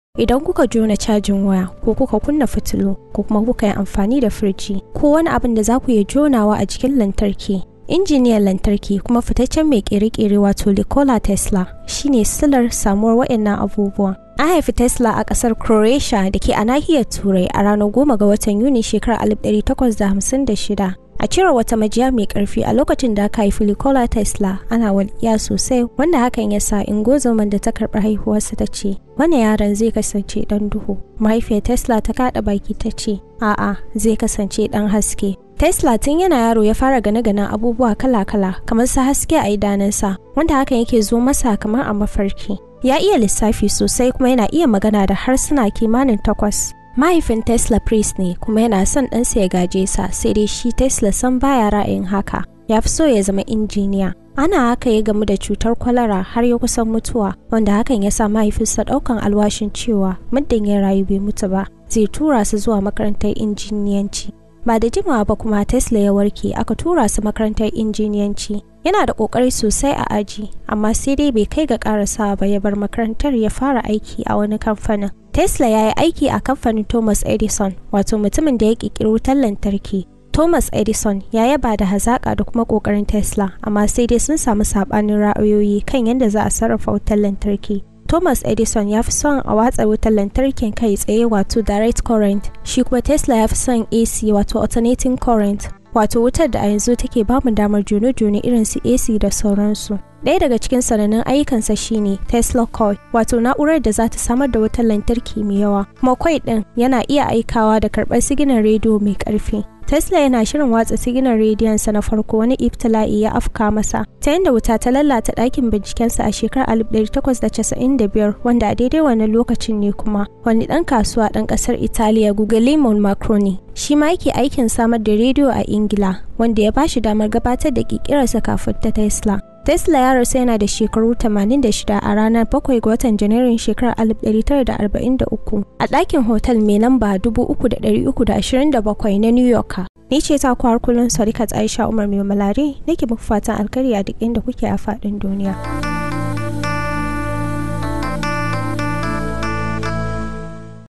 Tarihin Nikola Tesla
A haifi injiniyar lantarki, fittacen mai kere kere Nikola Tesla a garin Smiljan da a yanzu yake a matsayin kasar croatia a Turai a ranar 10 ga watan yuli shekarar 1856.